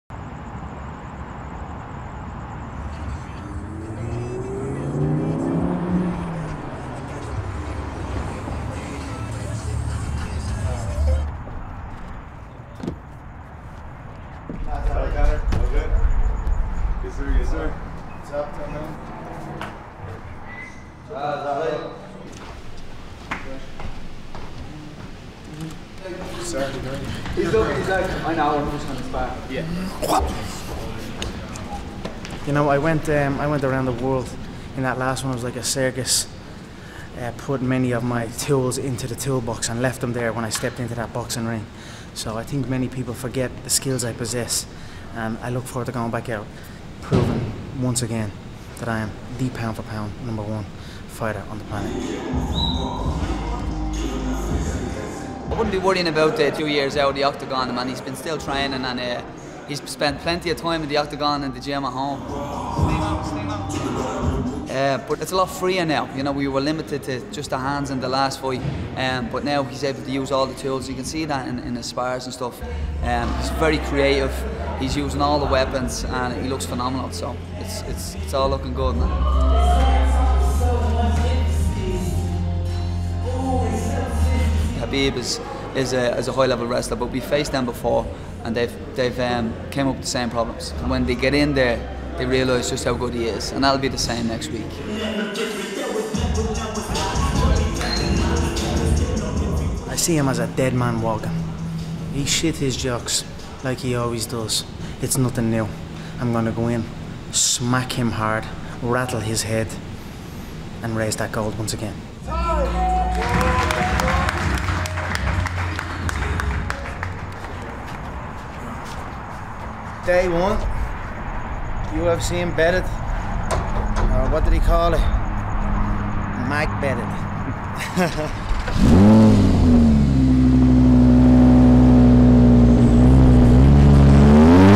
Brutal transformación física de McGregor I MARCA
El luchador irlandés prepara su regreso a la UFC dos años después de su 'retiro' con un nuevo método de entrenamiento, más rápido y letal que nunca...